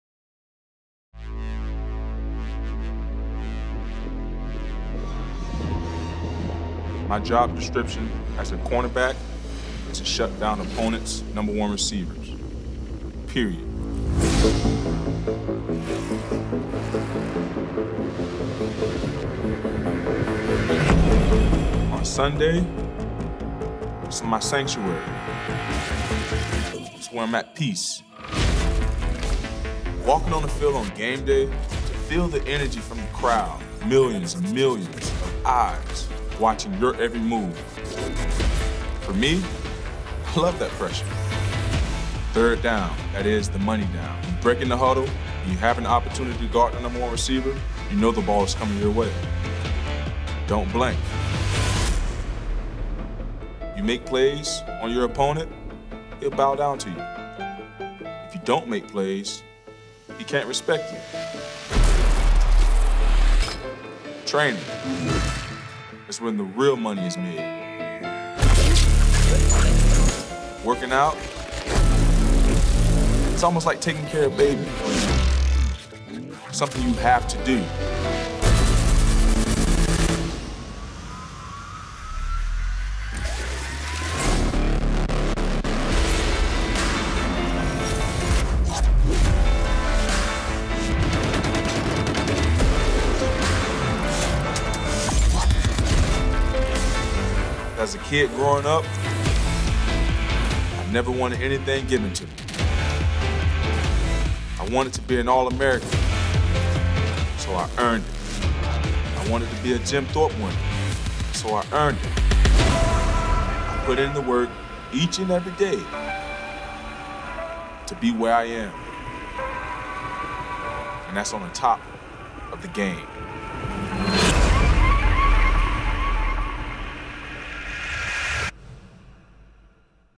EARN YOUR ARMOUR | Patrick Peterson
How did Patrick Peterson get to the top of the game? He put in work each and every day—and he’s not letting up any time soon.